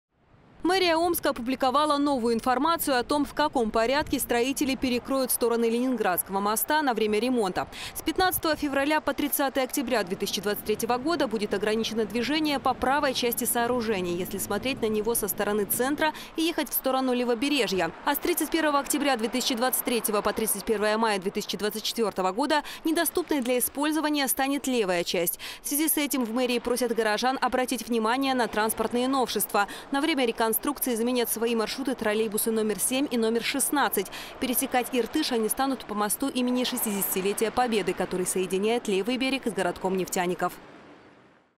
Внимание на карту
Мэрия Омска опубликовала новую информацию о том, в каком порядке строители перекроют стороны Ленинградского моста на время ремонта. Подробности далее.